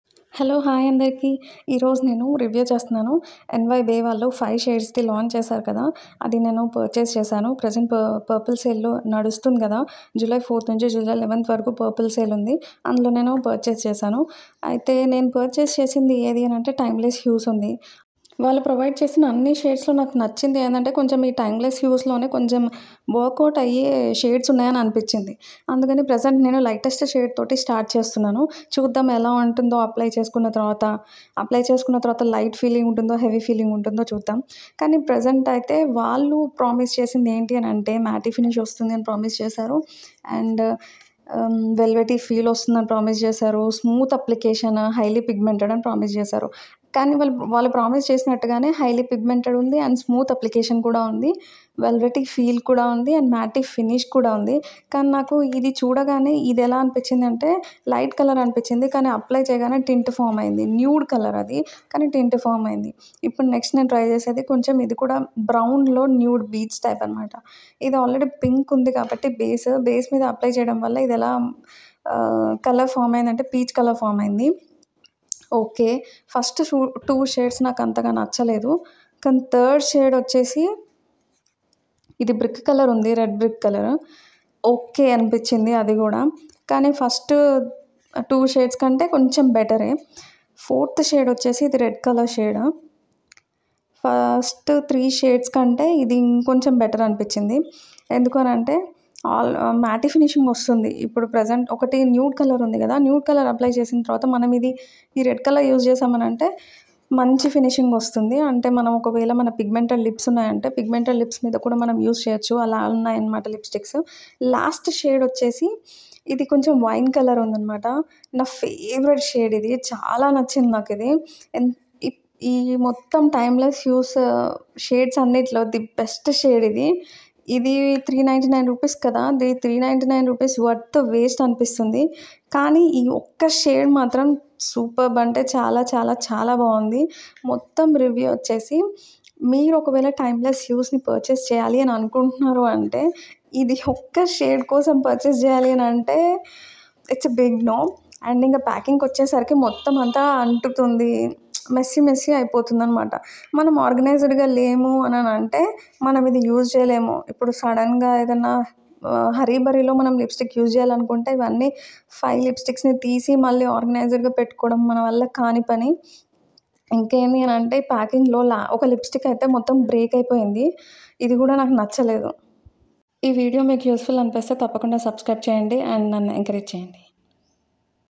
my honest review on NY bae lipstick 💄 5 in 1🤔adrishta
my honest review on NY bae lipstick 💄 5 in 1 shades 😐

I bought this lipstick from purplle sale and found it very very unique, the thing is the lipsticks are so creamy in texture, highly pigmented, smooth application and long lasting experience but what I felt is due to the creamy texture it feels heavy on lips not only that but lips getting dry due to highly pigmented property other wise the lipsticks are so good and the shades are unique🤗 you can buy it if you are a lover of creamy and smooth texture lipstick 😍💄

product link

NY Bae 5 in 1 Lipstick - Timeless Hues 03 (6.5 g) | Universal Shades | Vit E & Argan Oil | Matte Finish | Smooth Application | Multipurpose | Travel Friendly

note : this review is not to hurt anyone and not even spreading negatively but I just shared my opinion